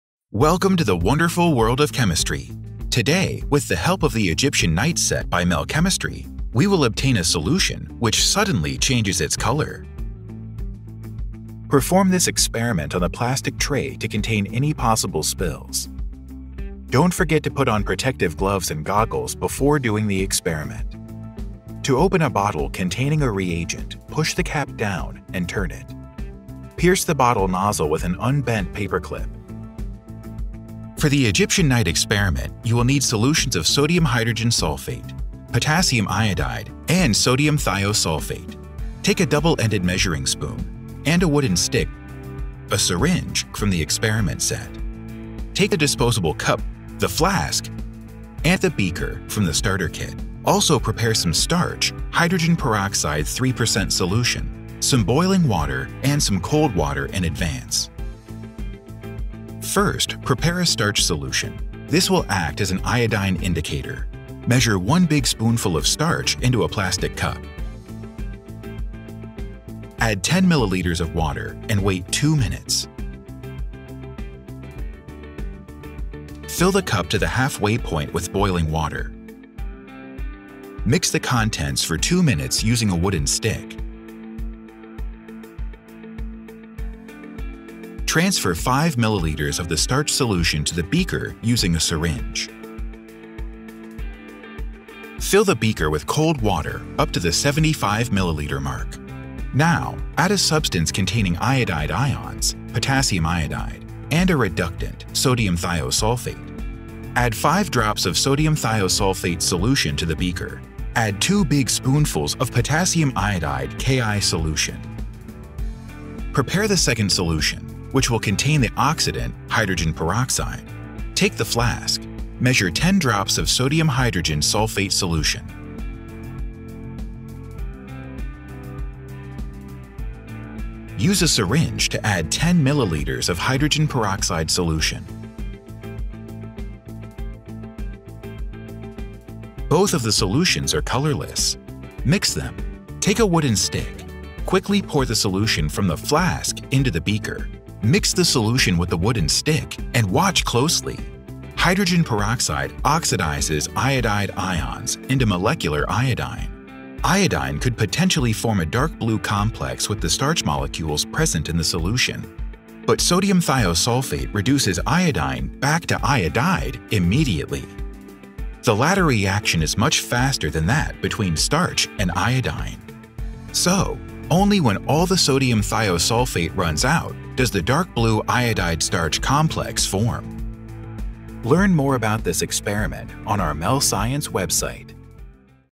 "Egyptian night" experiment from the "Egyptian night" set (UK edition)
Safety
* Put on protective gloves and eyewear.
* Conduct the experiment on the plastic tray.
* Observe safety precautions when working with boiling water.

Disposal
Leave the solution for 1 h, then mix the contents using a wooden stick. Add 10 drops of Na₂S₂O₃ and mix again. Pour the solutions down the sink. Wash with an excess of water. Dispose of solid waste together with household garbage.